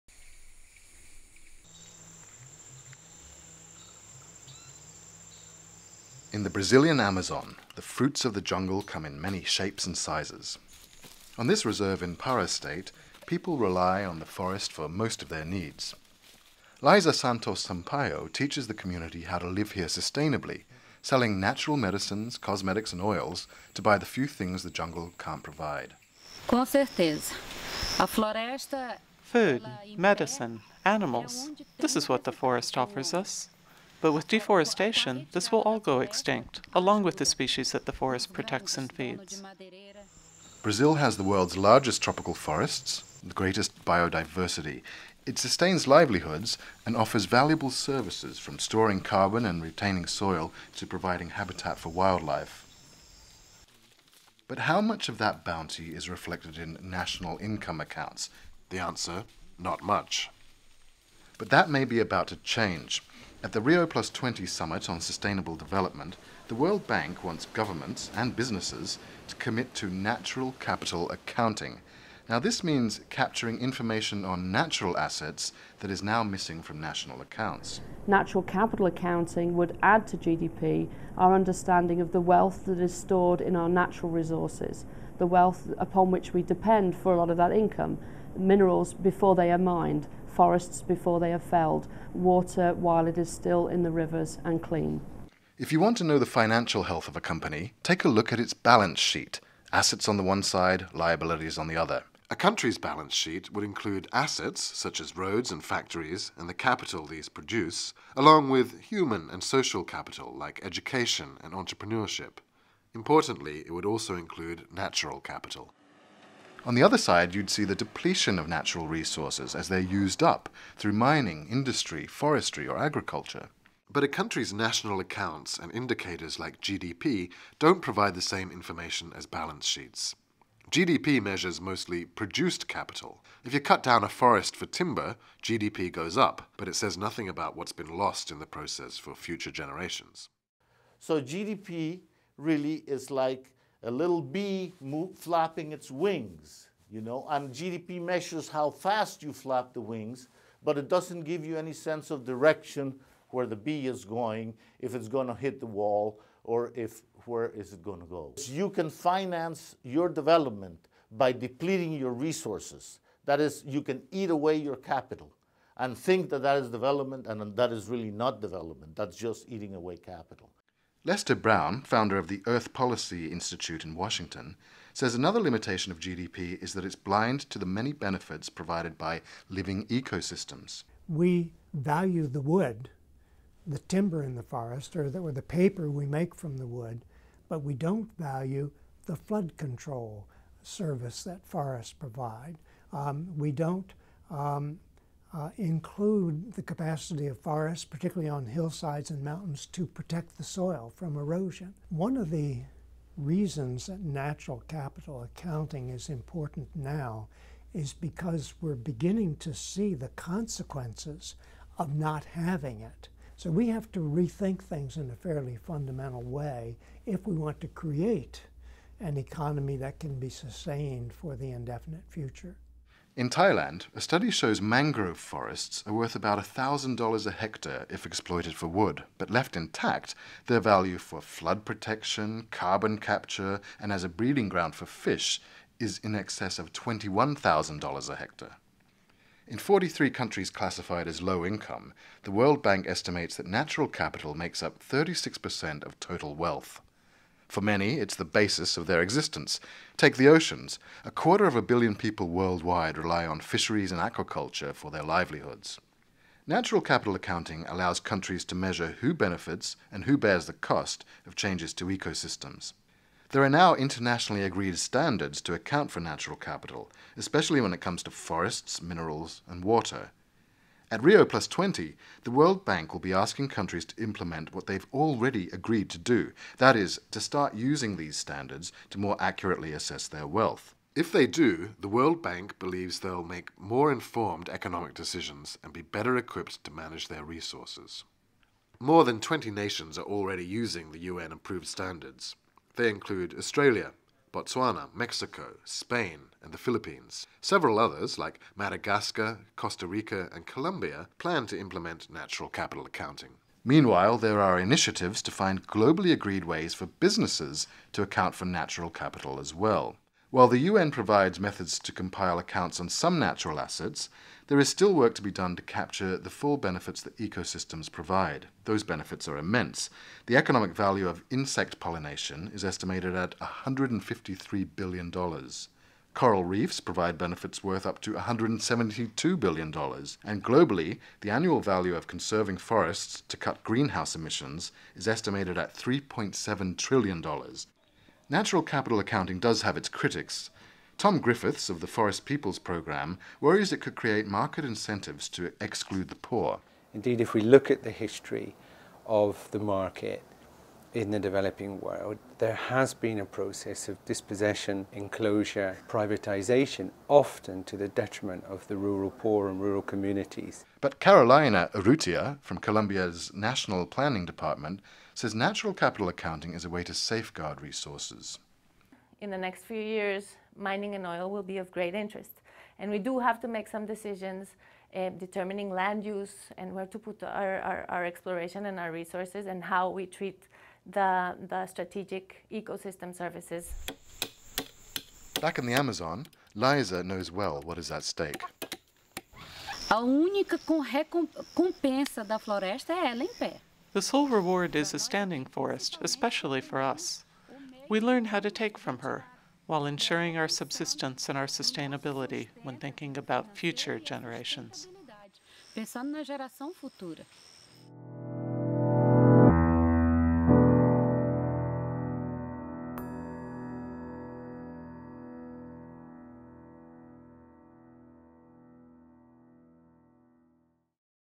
Time to Account for Natural Capital
If you want to know what the financial health of a company is, you look at its balance sheet: assets on one side, liabilities on the other. A country's balance sheet would include assets such as roads and factories, along with human and social capital like education and entrepreneurship. Importantly, it would also include natural capital. On the other side, you'd see the depletion of natural resources as they're used up -- through mining, industry, forestry, agriculture. Rio+20 offers a unique chance to include `natural capital' in national accounts and move towards development that is more sustainable.